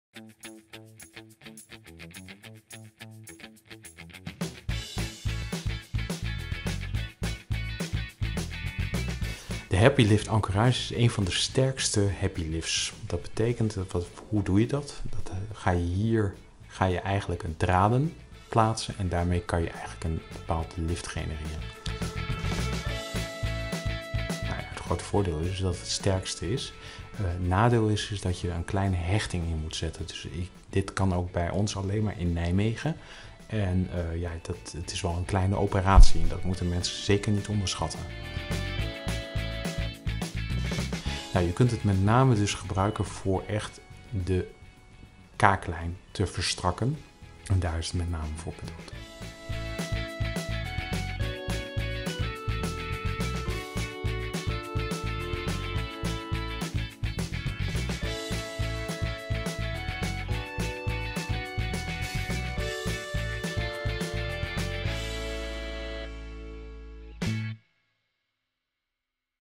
Happy Lift Ancorage (draadlift) | Praktijk voor Injectables
Happy Lift Ancorage is een behandeling met speciale draden. Wat zijn de voor- en nadelen en waar kun je dit middel gebruiken? Dokter Meulenaar vertelt het in deze video!

Telefoon: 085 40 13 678